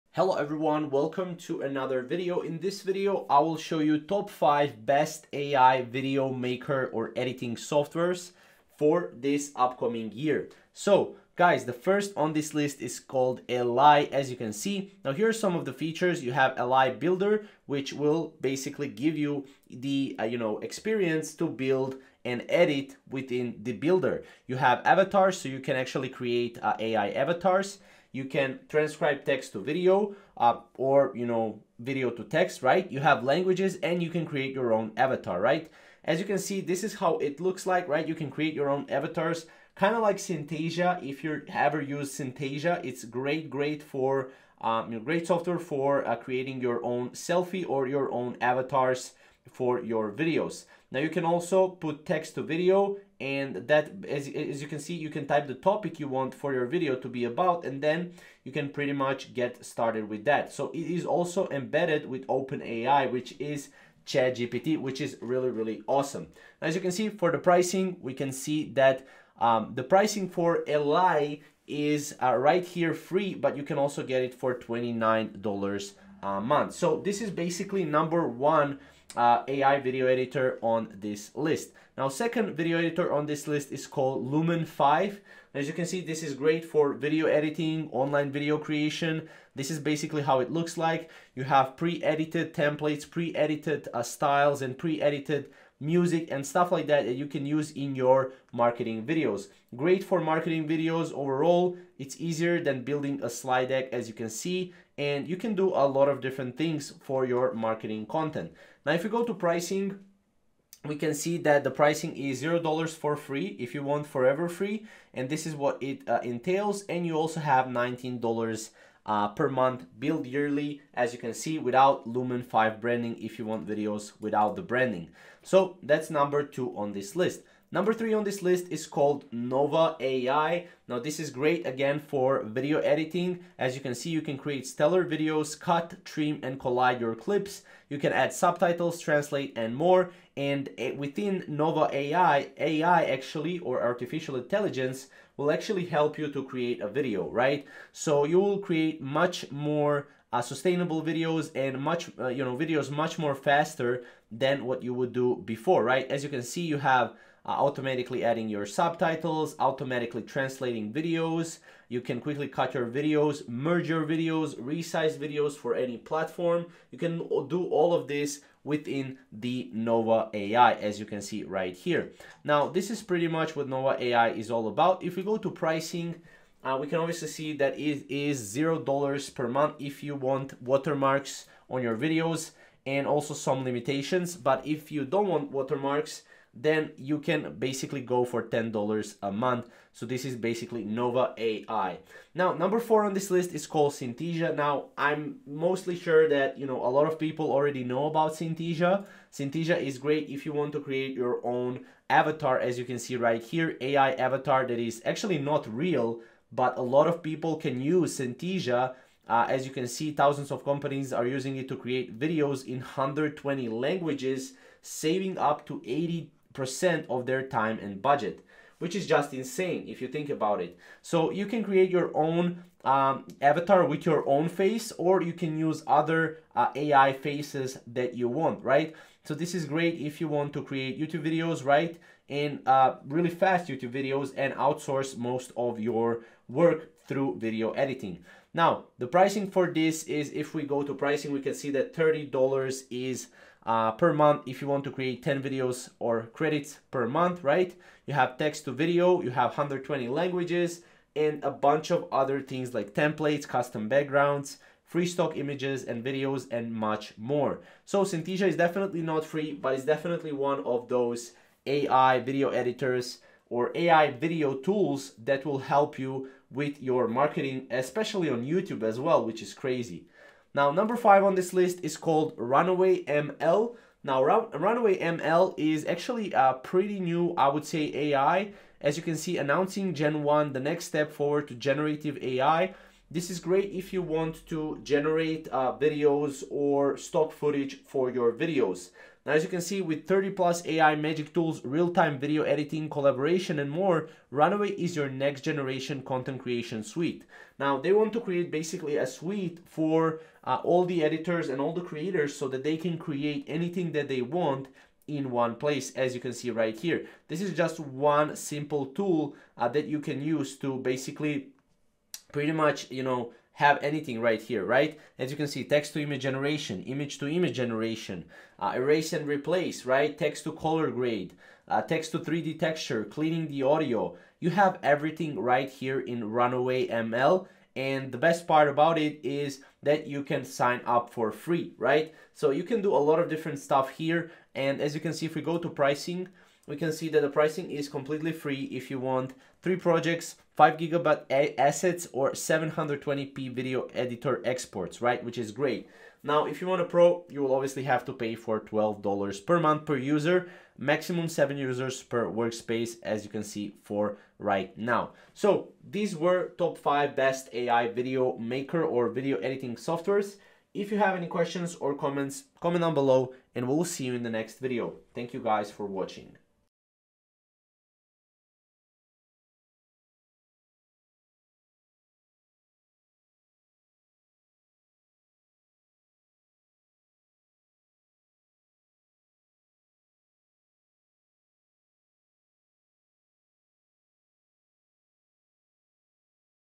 5 Best AI Video Maker/Editing Software (2024)
5 Best AI Video Maker/Editing Software (2024)

In this video, I will show you 5 Best AI Video Maker/Editing Softwares